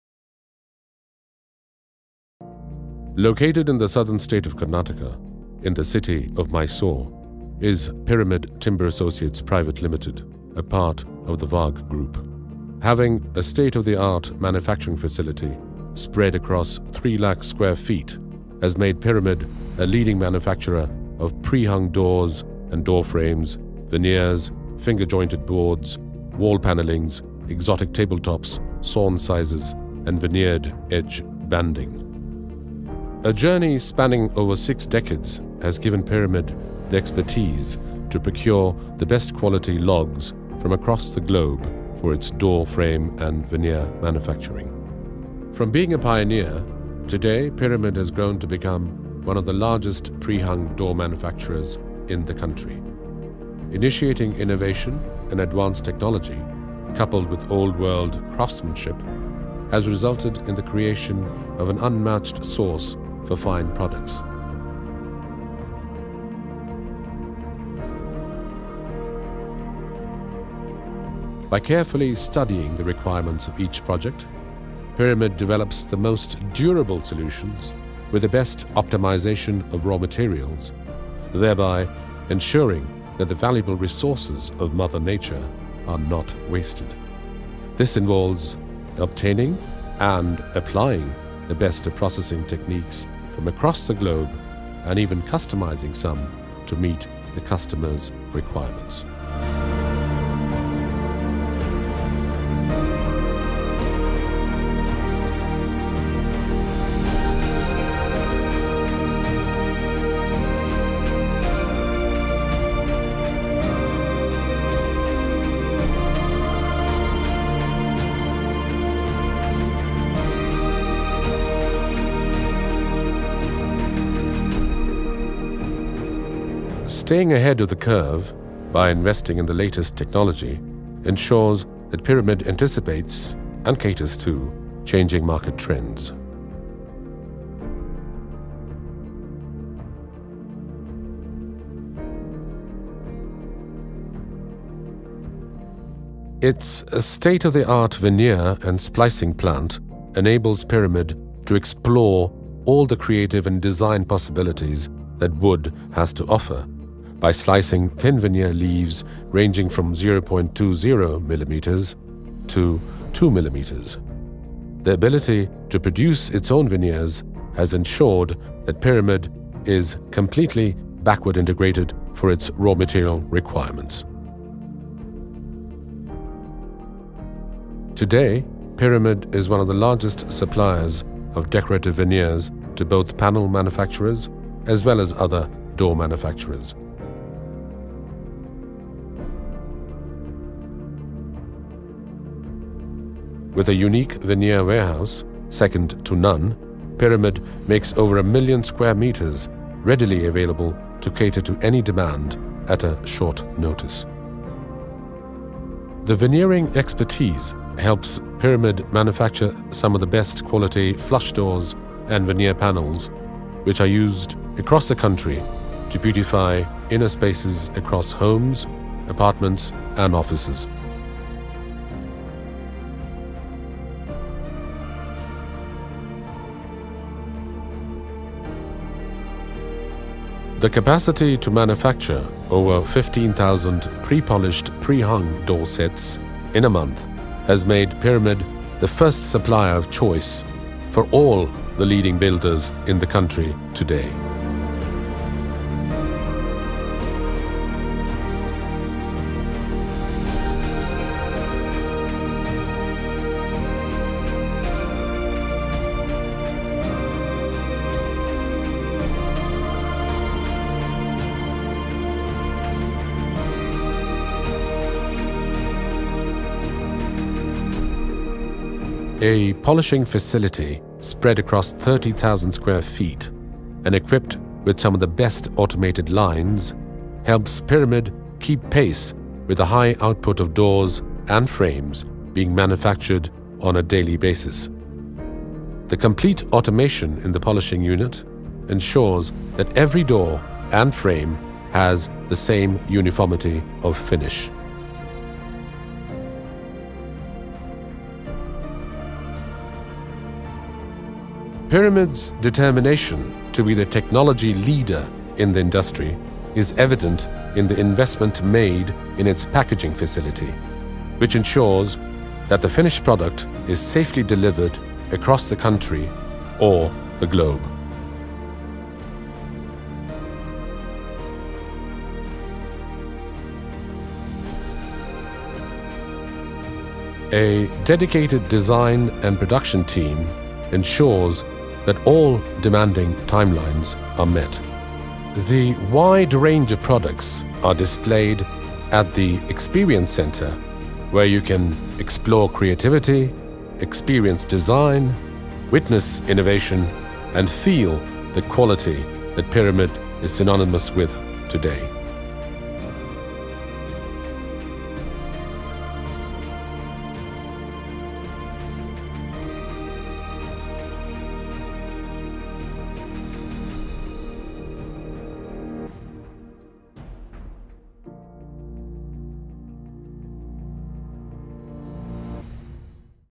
Pyramid Timber Factory
Located in the beautiful lush green surrounds of Mysore in the southern state of Karnataka is Pyramid Timber Associates Pvt. Ltd. Established in the year 1987, Pyramid Timber is engaged in the manufacturing of Wooden Doors, Door Frames, Decorative Veneers, Finger Jointed Boards, Wall Panelling’s, Sawn Sizes, Thermo Wood, Veneer Edgebanding and Exotic live Edge Table Tops.